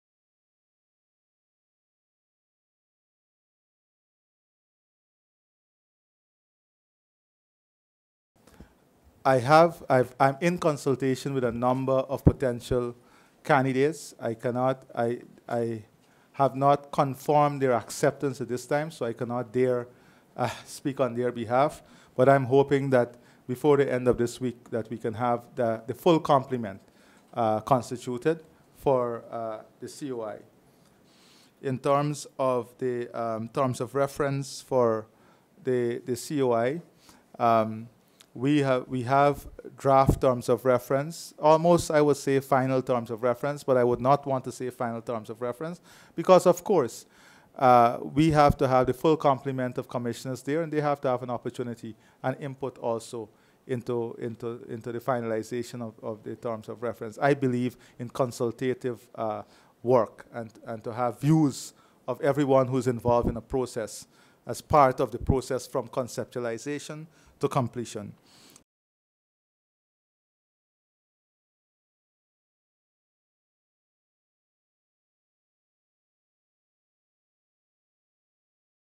MAHDIA TRAGEDY: INQUIRY TEAM NEAR FINALISED - PRESIDENT ALI
The members of the Commission of Inquiry (CoI) into the deadly fire at the female dormitory of the Mahdia Secondary School in Region Eight are near finalised, President Dr. Irfaan Ali said on Wednesday. Previously, the President said Major General (Ret’d) Joseph Singh would lead the CoI into the fire that led to the deaths of 20 children, 19 girls and a five-year-old boy.